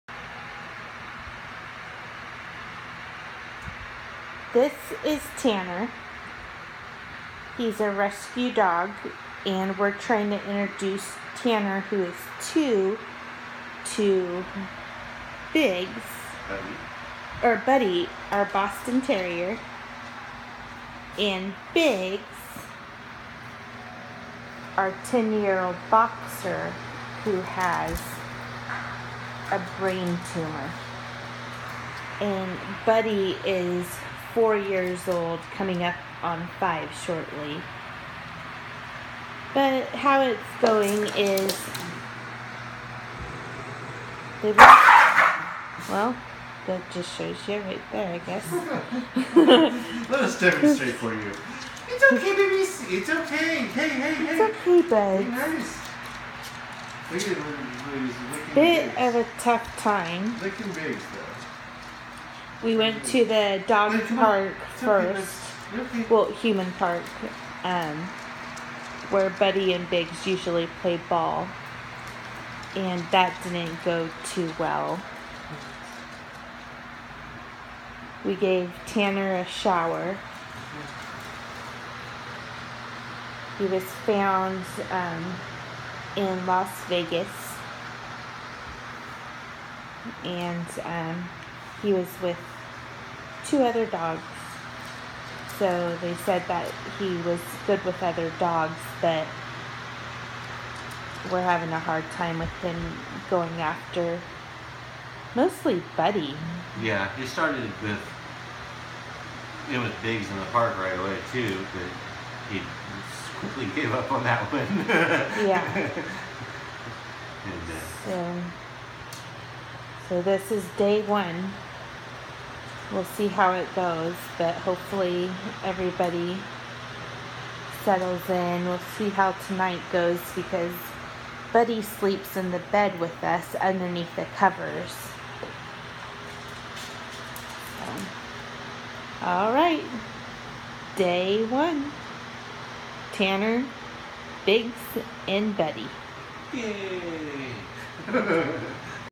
Dog introduction, doesn't go so well
This is the introduction of a rescue dog that has the boys fighting.